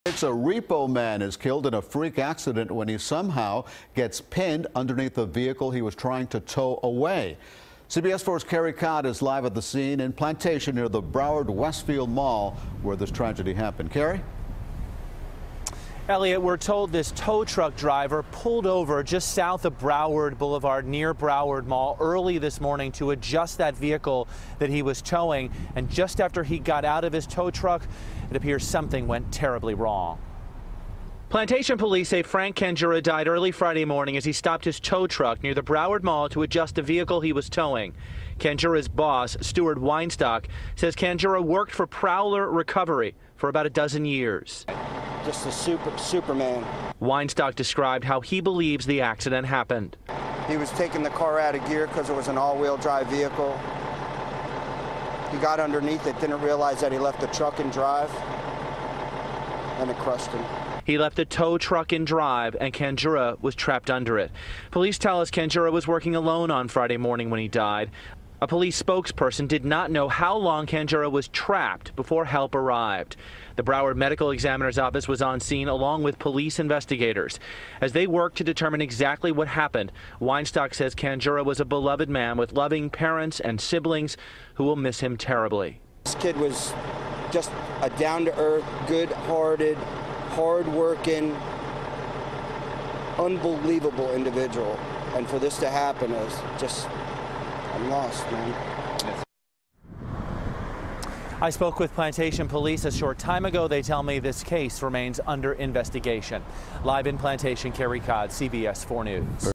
Tow Truck Driver Killed On The Job Near Broward Mall
CBS Miami's Carey Codd reports from Plantation.